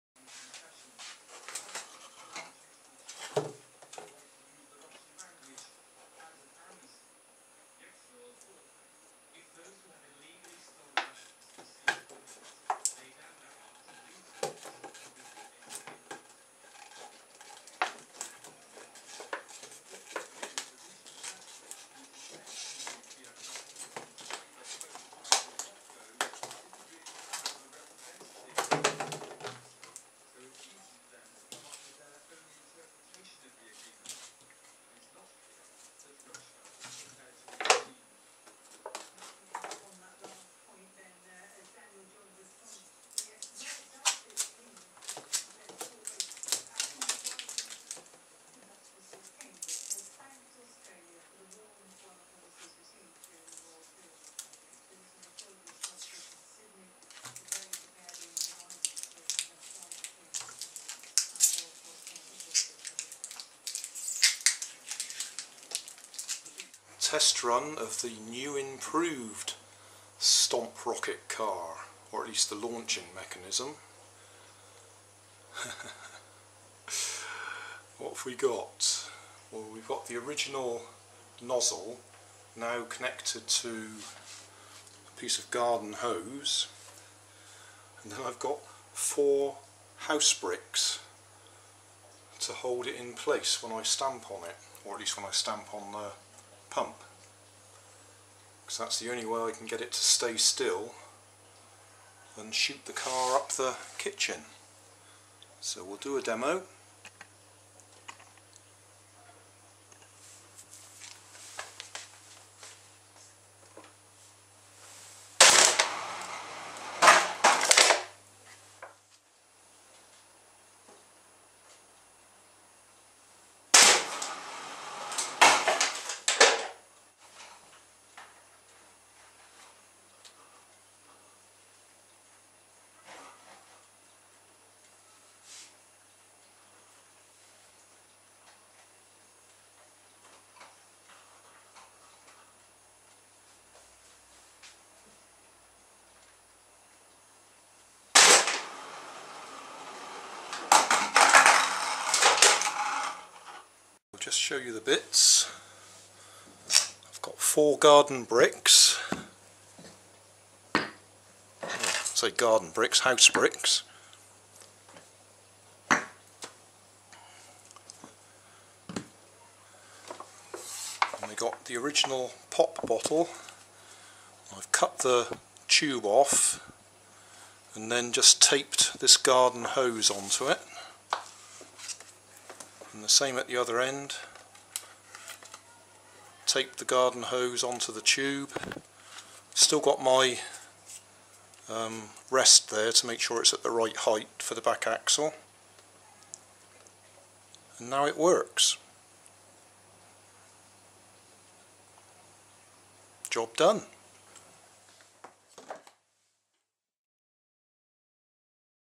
Pop Bottle Stomp Rocket Car Improved Launcher
I have improved the launch mechanism for my Pop Bottle Stomp Rocket Car.

Additional Components
Garden Hose
4 house bricks

Main Tools
Utility knife

Here is a playlist with related videos including the Easter Egg Pop Bottle Stomp Cannon and my Pop Bottle Stomp Cannon.